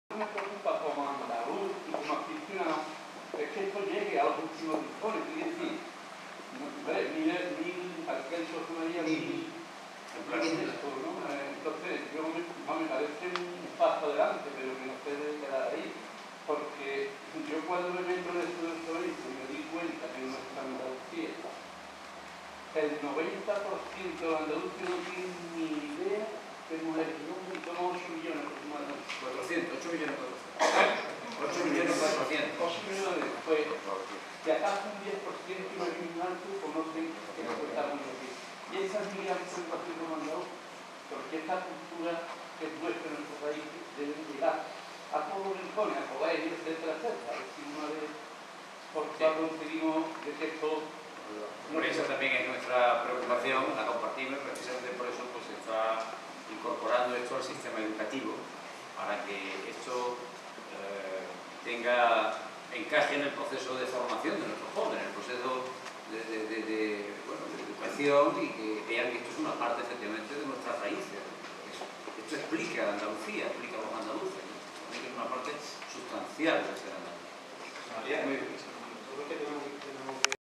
Manuel Vallejo: Presentación de su Completa Discografía 3/4
Paulino Plata,consejero de cultura de Andalucía nos trae al fin!!! la magna antología digitalizada del inigualable cantaor de la calle Padilla...